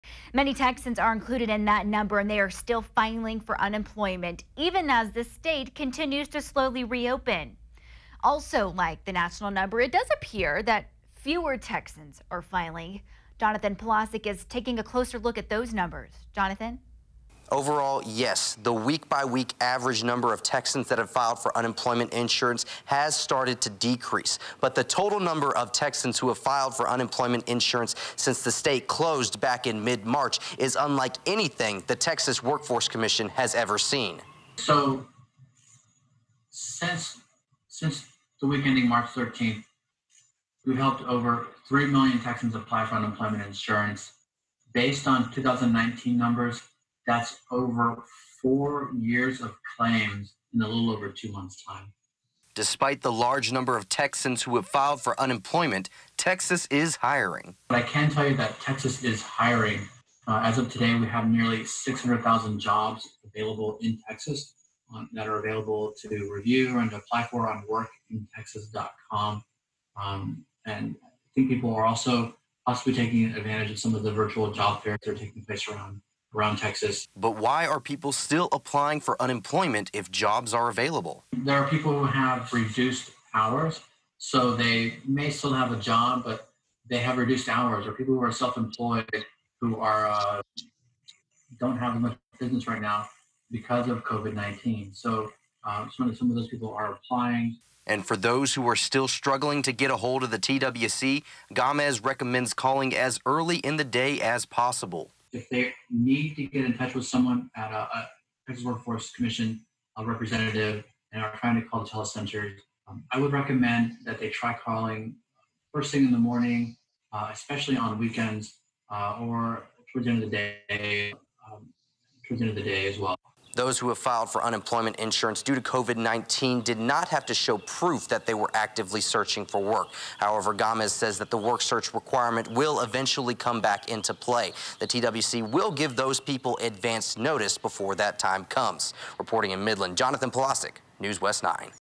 The number of Texans filing for unemployment is slowly decreasing
As the state continues to do open up so do more jobs, and Texans are starting to take advantage.